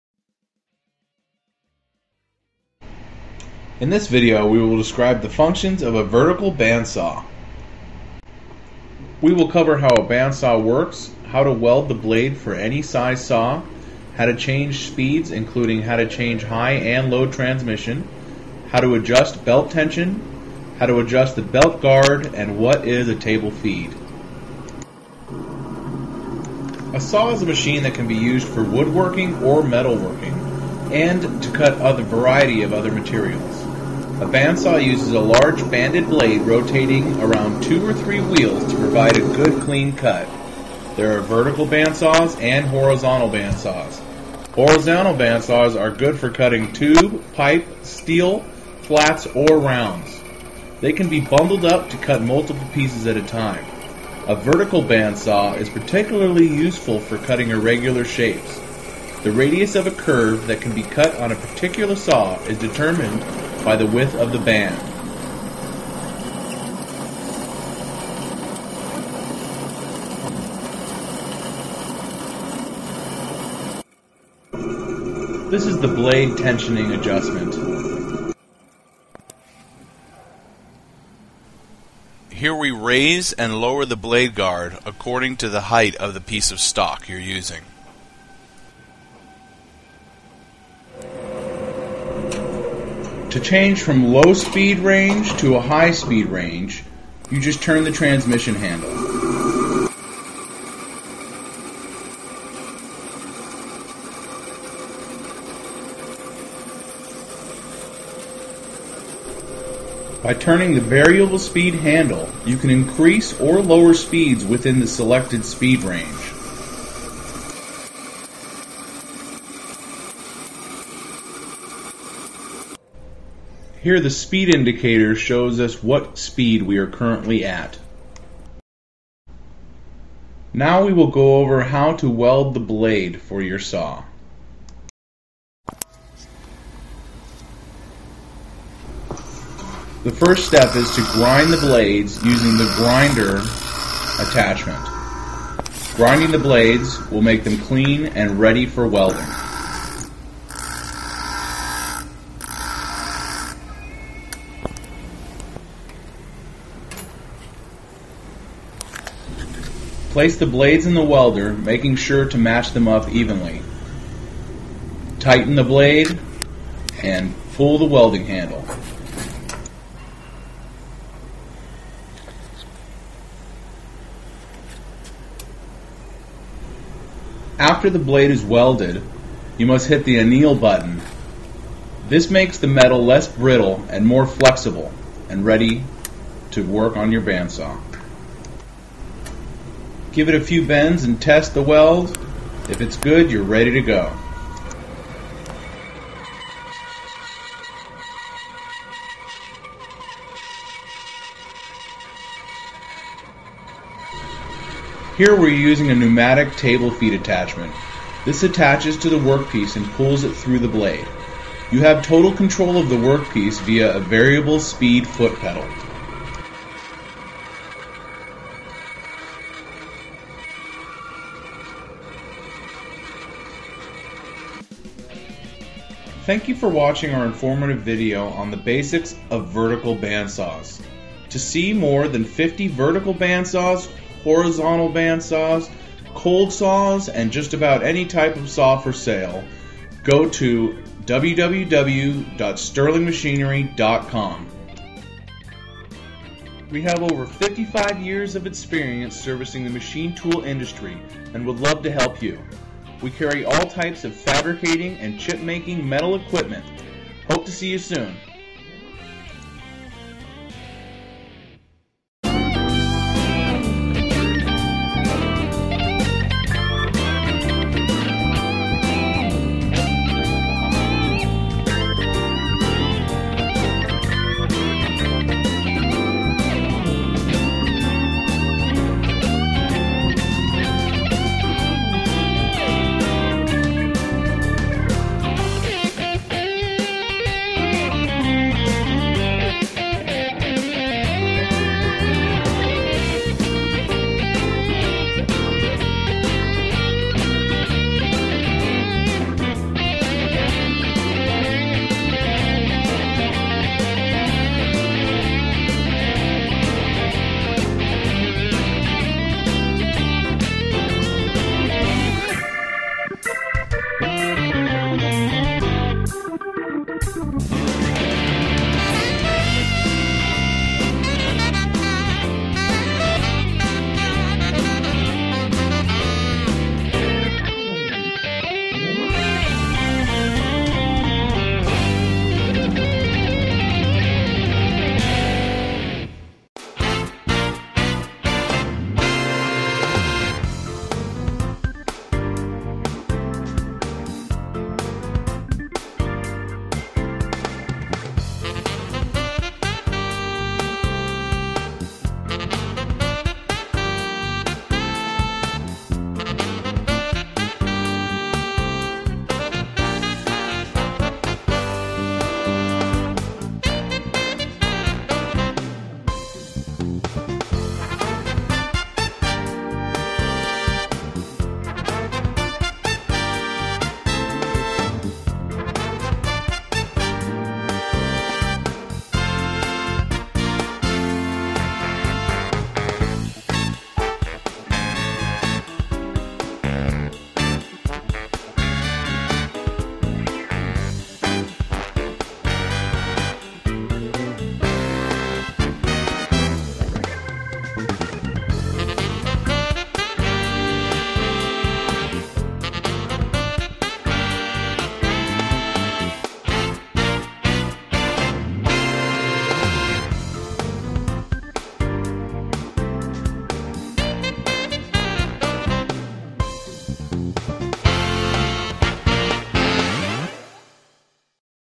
How To Use a Vertical Bandsaw Machine. Welding, Speed Ch...
This video will show you the basics on how to use a vertical bandsaw machine.  Here is a Doall vertical bandsaw that has a 2 speed transmission, variable speed pulleys, blade welder and grinder, and a pneumatic table feed.